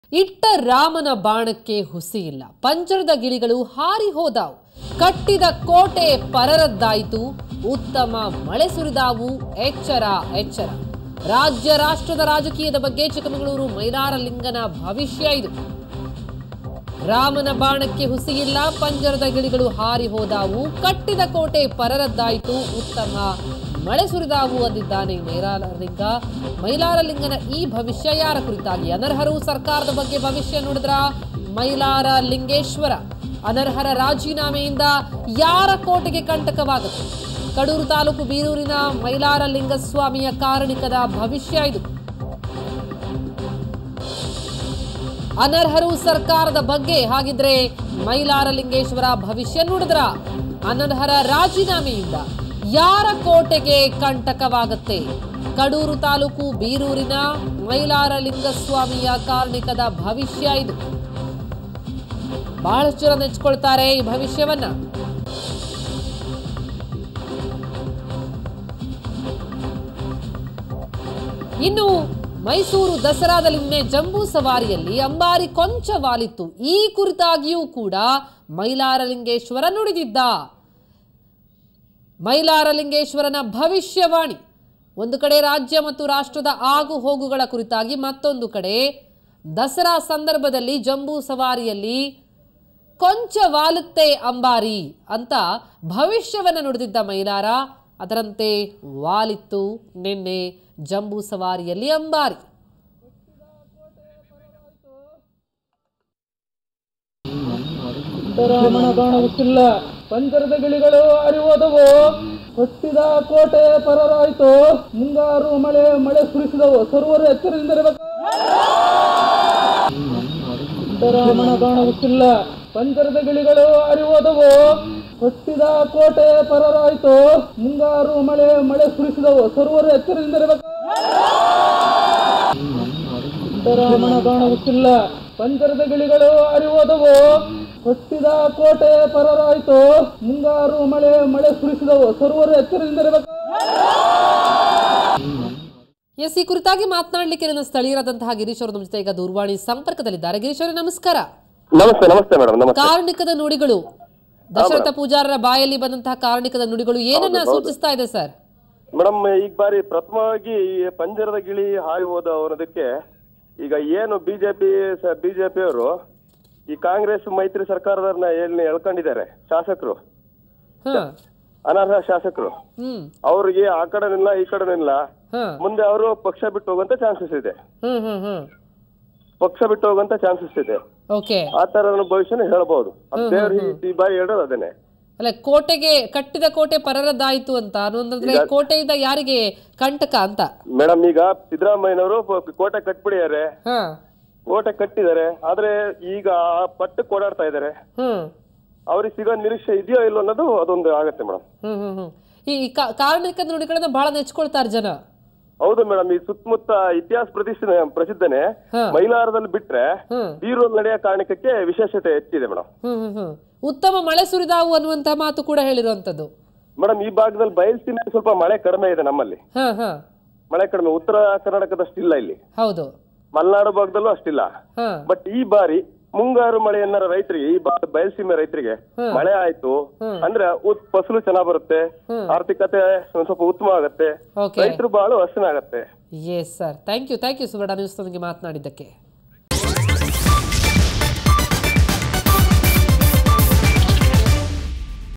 Mylara Lingeshwara Karnika Predicts Heavy Rains & Political Instability In State
Chikkamagaluru'; Mylara Lingeshwara Karnika Predicts Heavy Rains & Political Instability In State